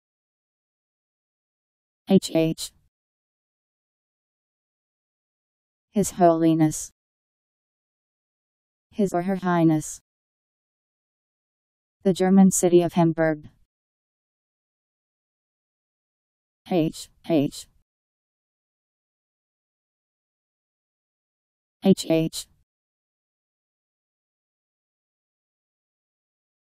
HH Meaning
Video shows what HH means. His Holiness.. His or Her Highness.. . The German city of Hamburg..  HH Meaning. How to pronounce, definition audio dictionary. How to say HH. Powered by MaryTTS, Wiktionary